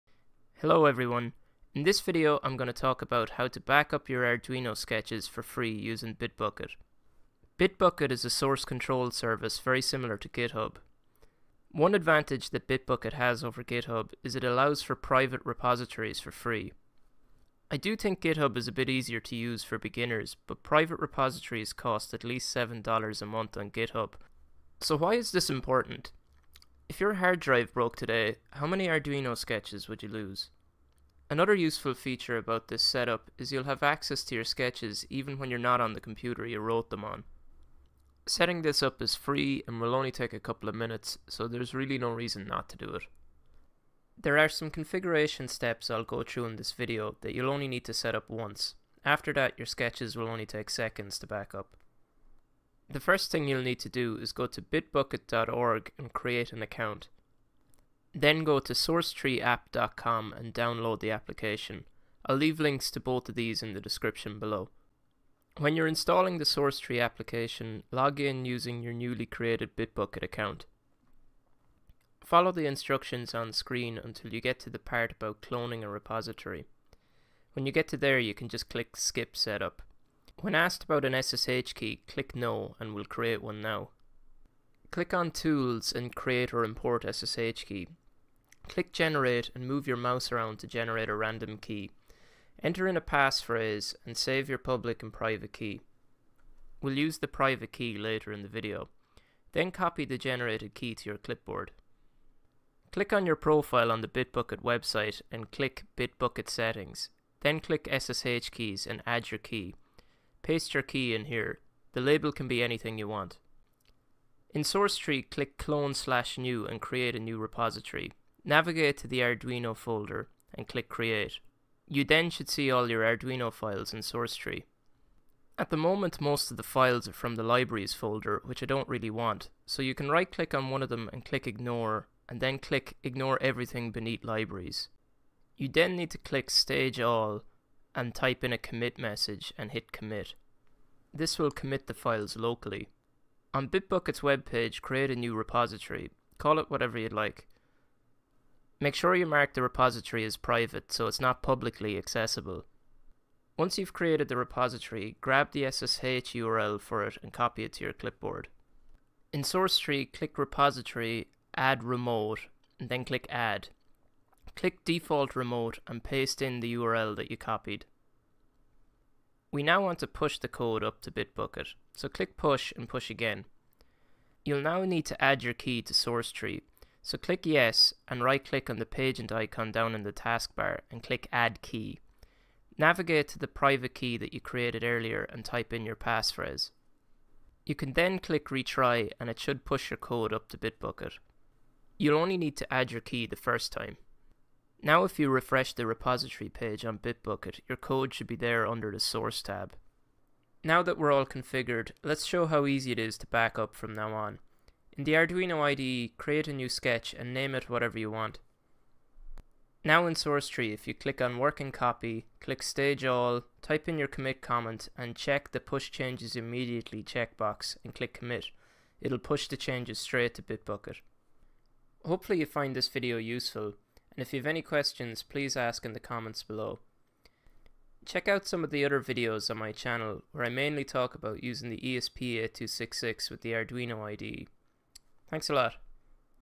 How to backup Arduino sketches for free using Bitbucket
Please excuse the croaky and more nasally voice, I was a little sick when recording!

As always, please let me know if you have any questions or feedback.